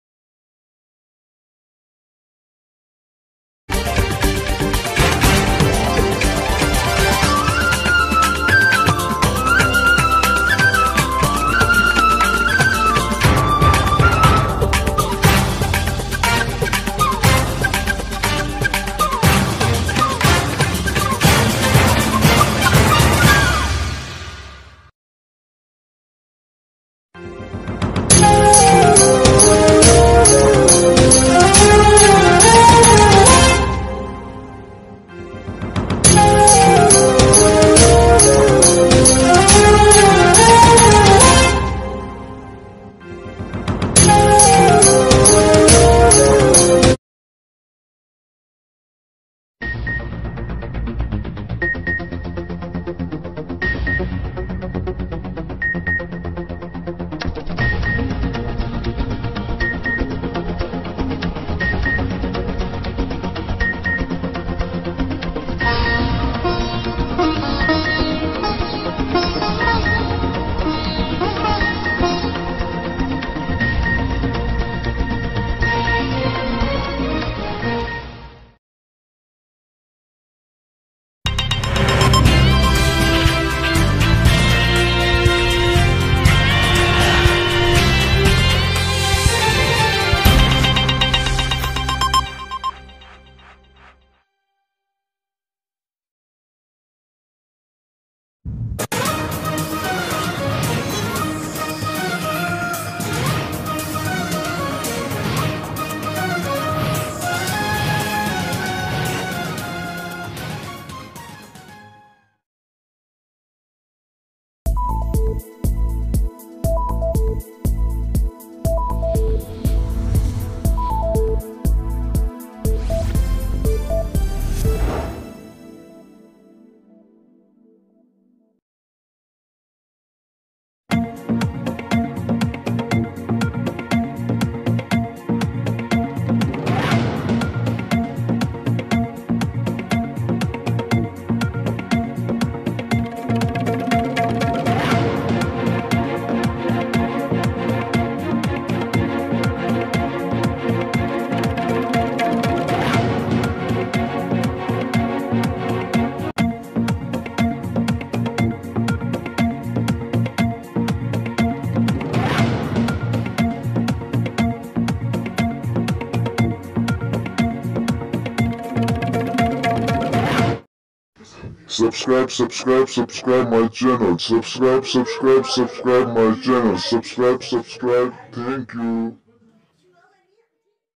Top 7 Pakistani News Channel and their background Music.
Pakistani News Channel and their background Music. thanks